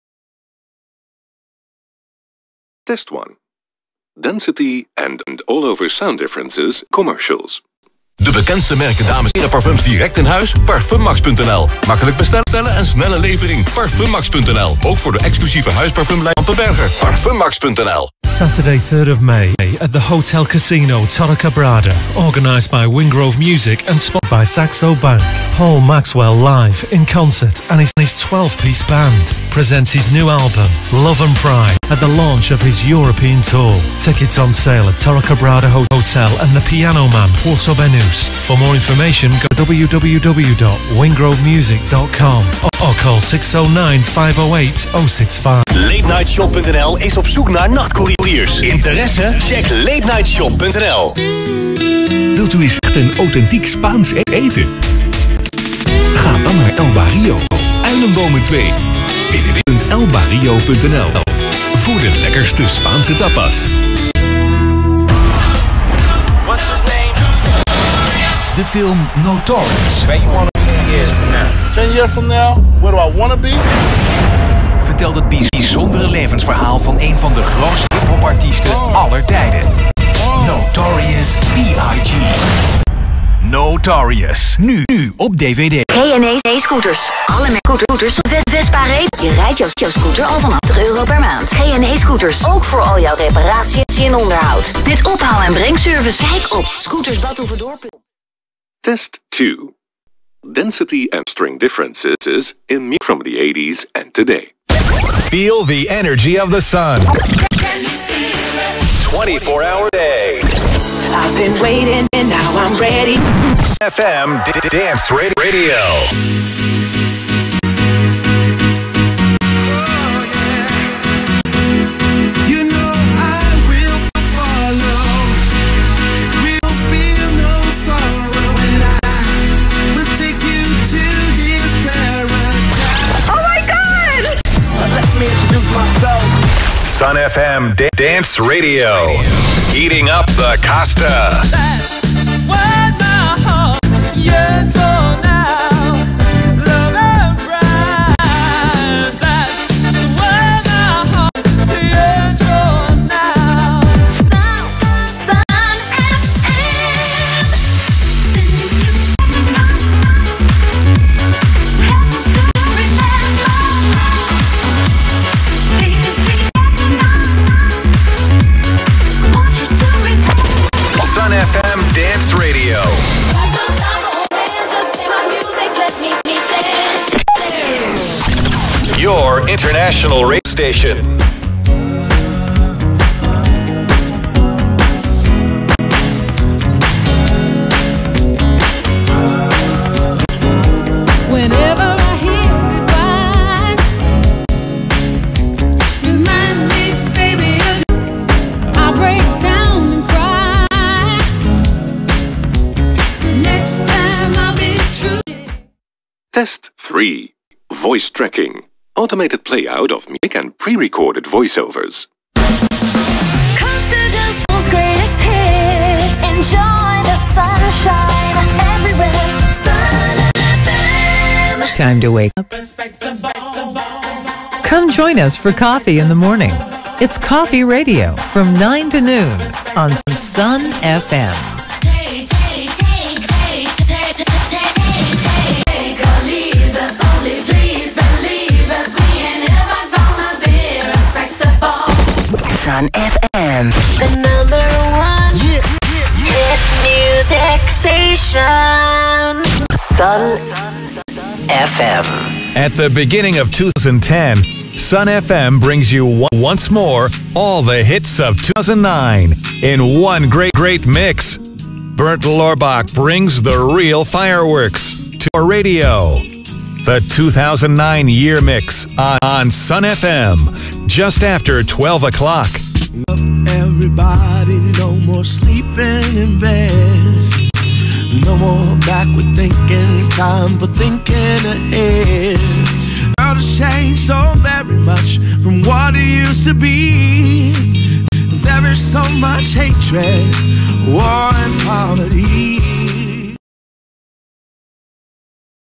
Breakaway One vs Optimod 8600
In this video, I put a $500 audio processor (Breakaway One) up against a $17,000 audio processor (Orban Optimod 8600).

The Optimod sounds great, very sharp and loud, however, Breakaway has a much more natural sounding output.